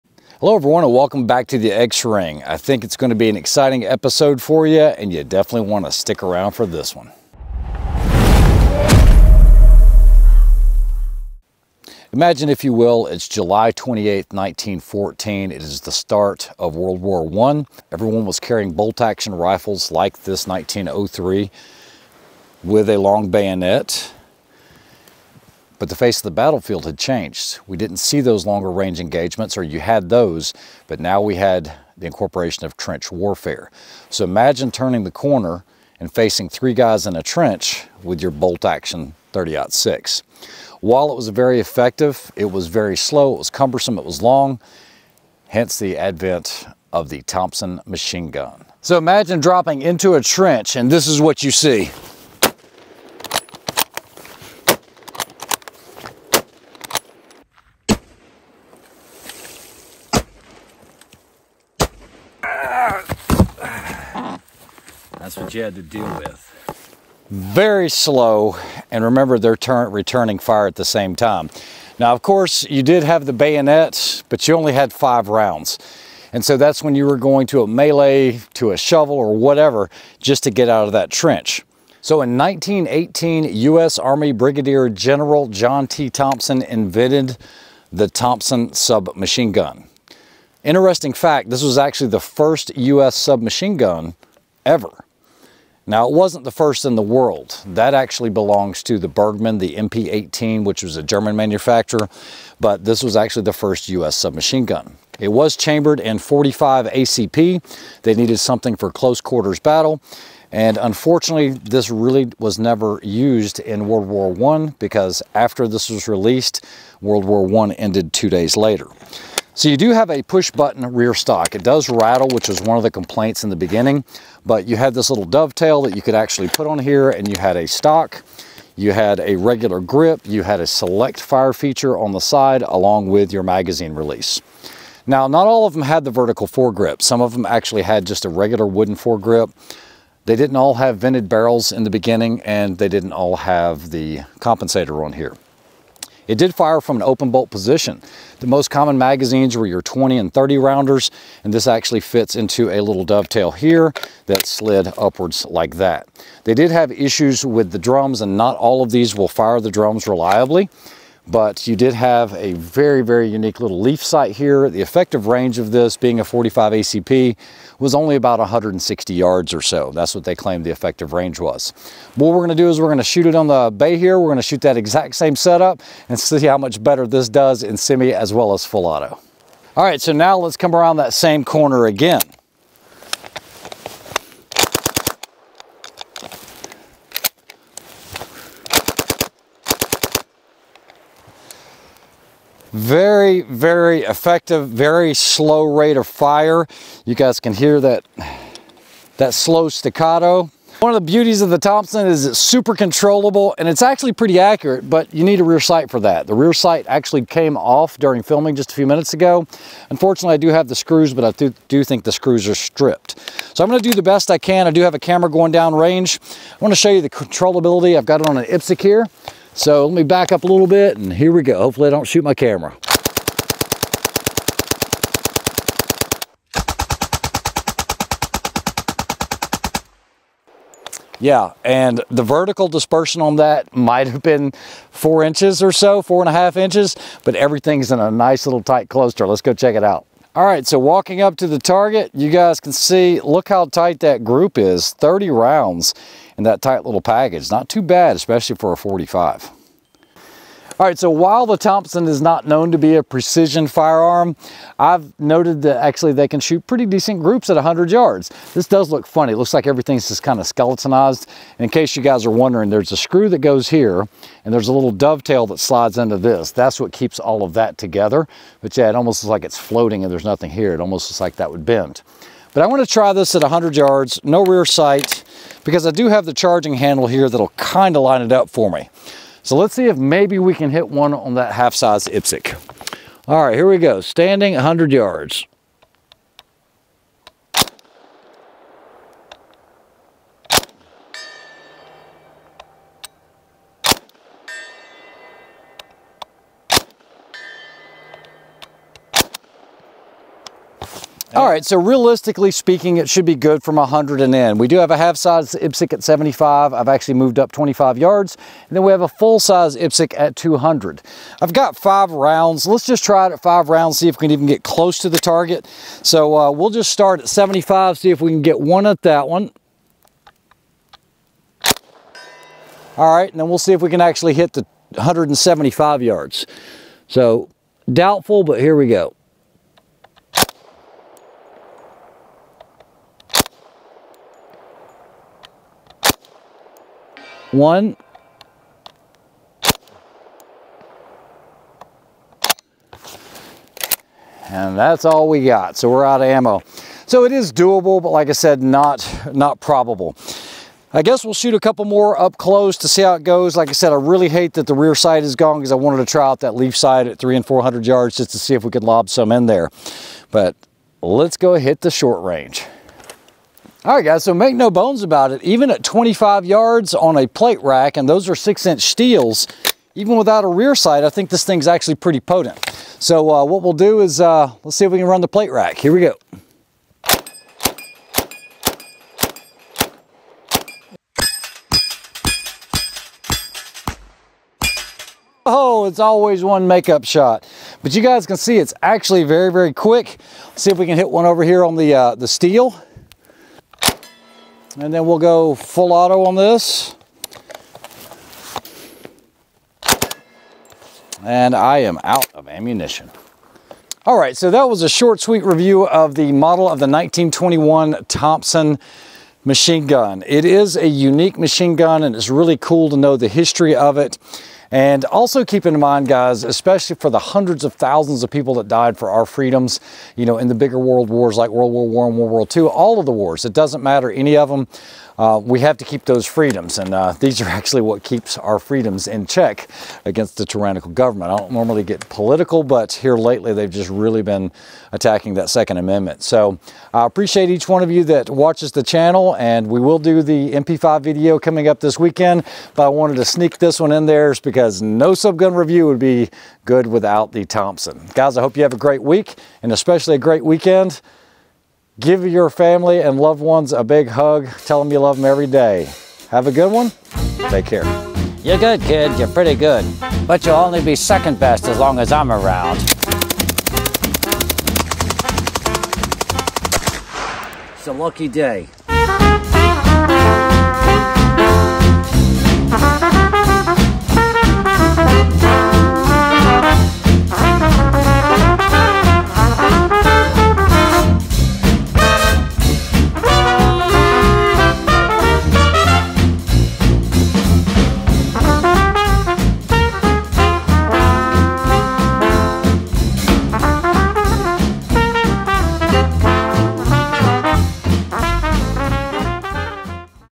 The Thompson Submachine Gun
Quick review of the Thompson Submachine gun by Auto-Ordnance.